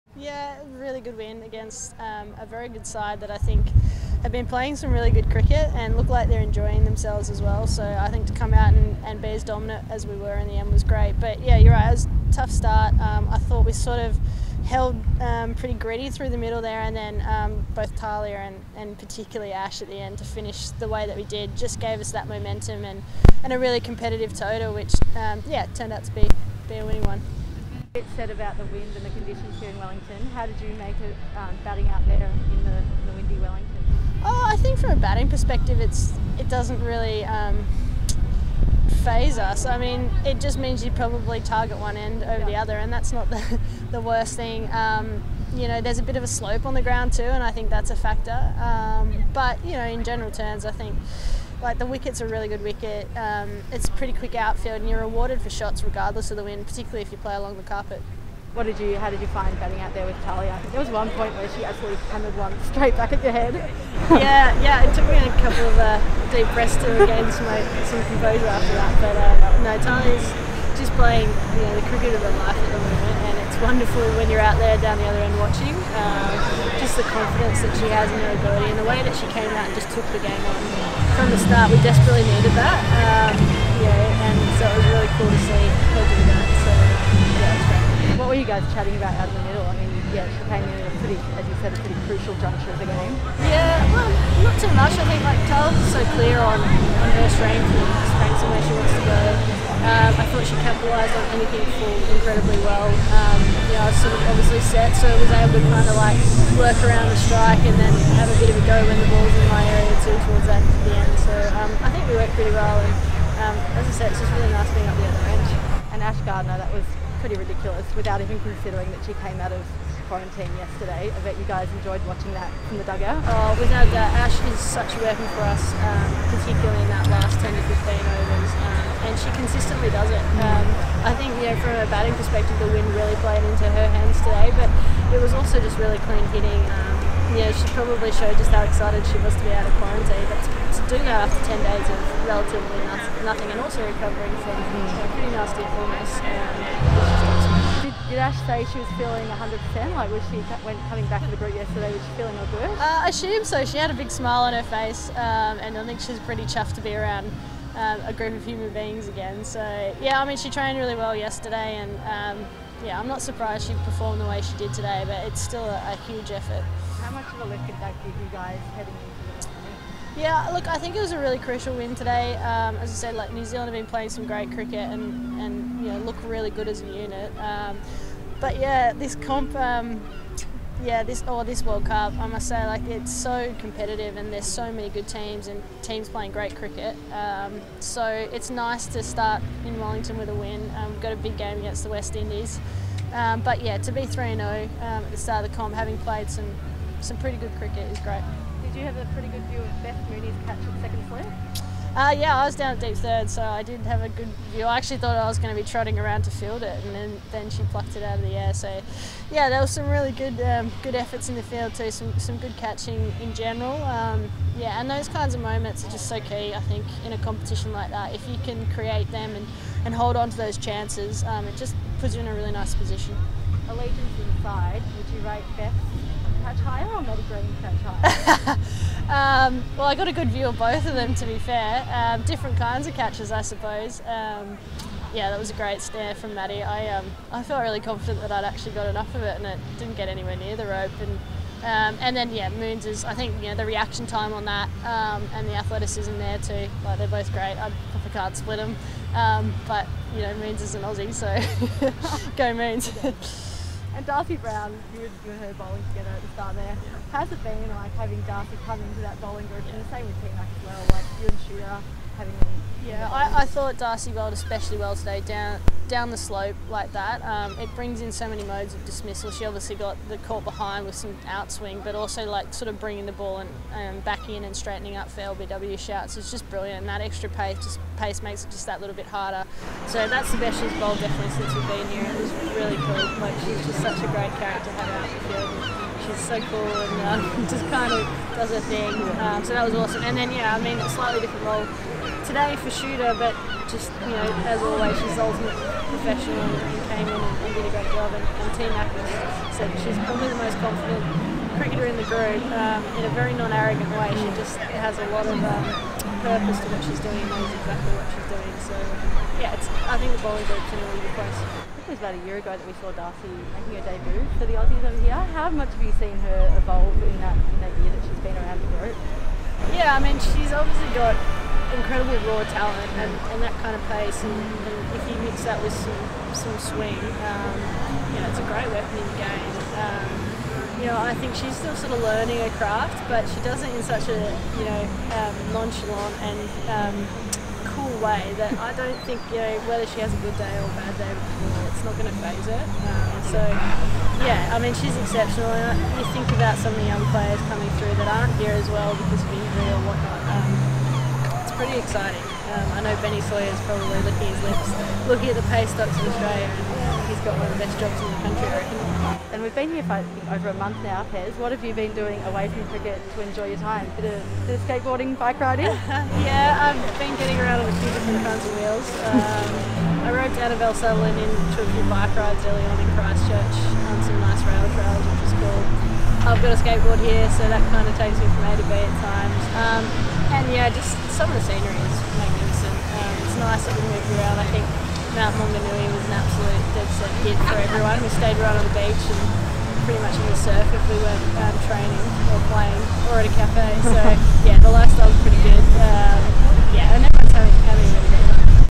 3-0! Perry reflects on 'dominant' Aussie's hot start to World Cup | ICC Women's ODI World Cup 2022
Following her gritty half-century and crucial dismissal of Sophie Devine, Ellyse Perry spoke to cricket.com.au about her partnership with Tahlia McGrath, Ashleigh Gardner's remarkable innings as well as a superb performance from Darcie Brown.